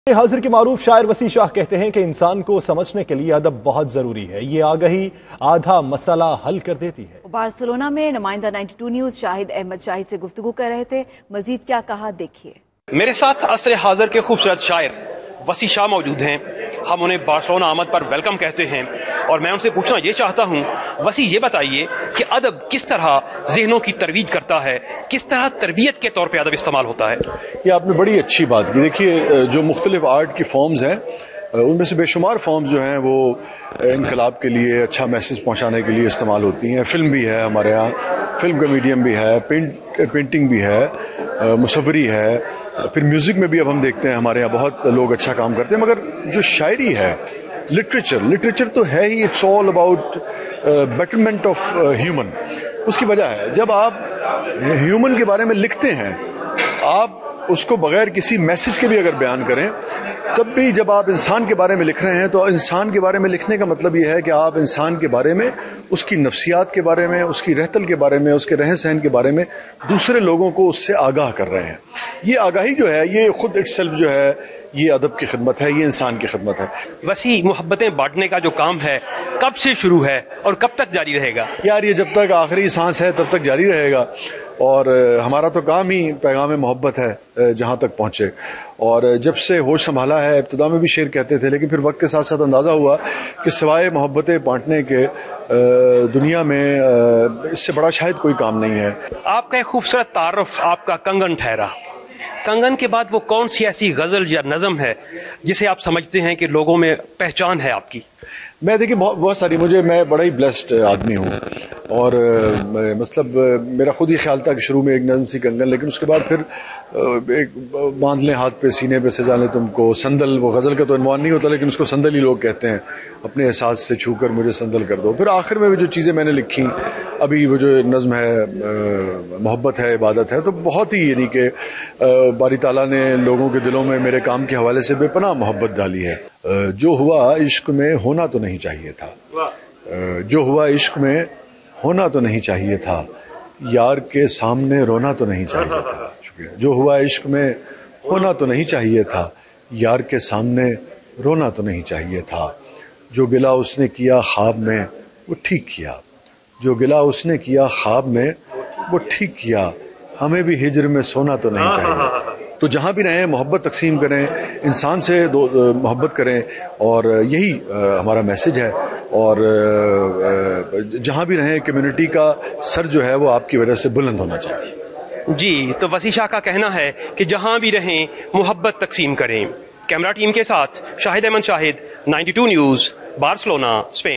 Poet Wasi Shah says literature is very important for human understanding | 92NewsHDUK
Poet Wasi Shah says literature is very important for human understanding